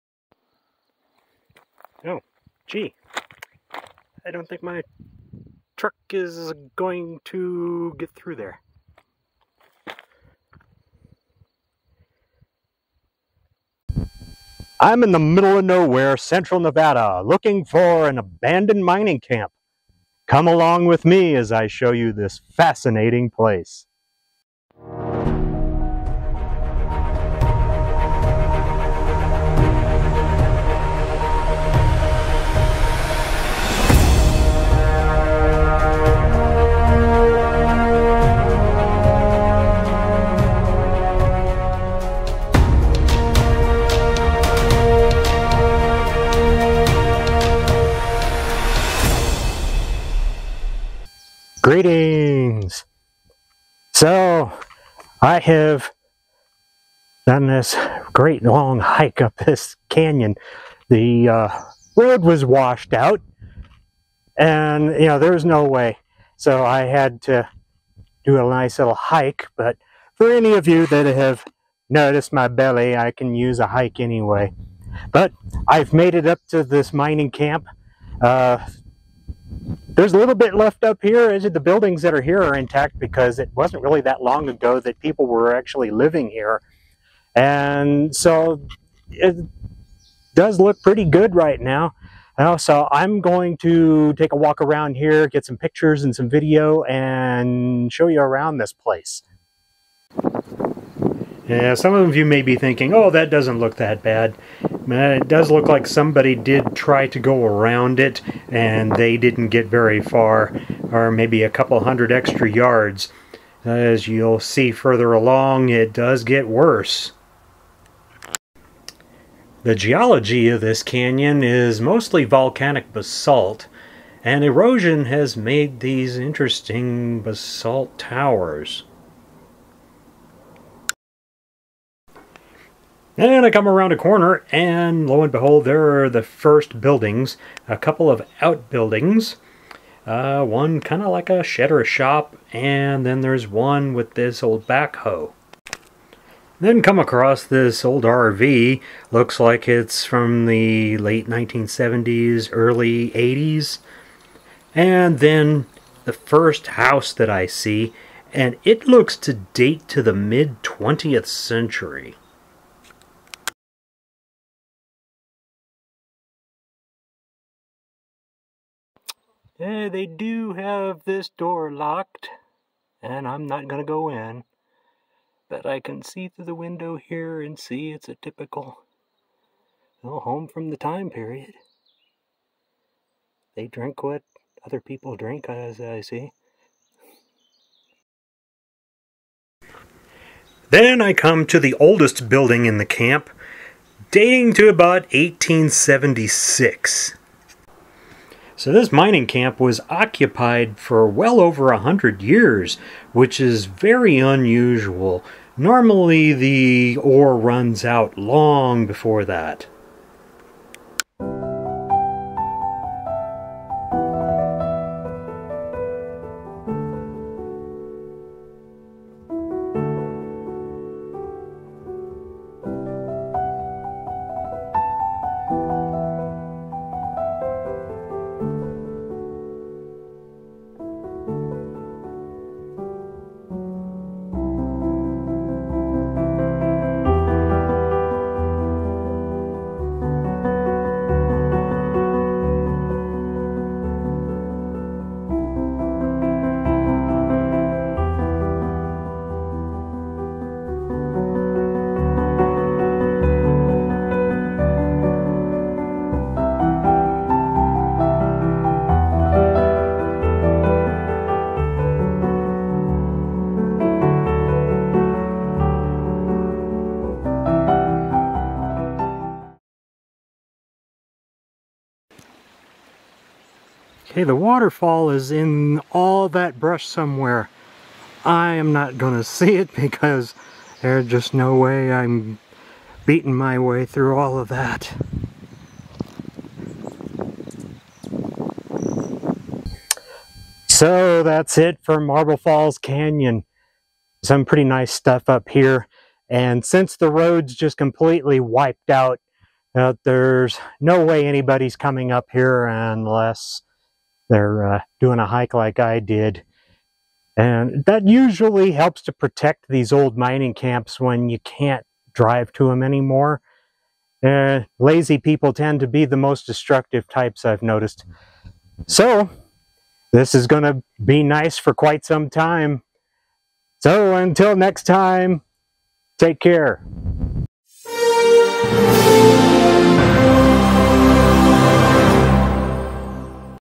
Abandoned Mining Camp
In a remote part of central Nevada, I'm in search of an old mining camp I saw on satellite.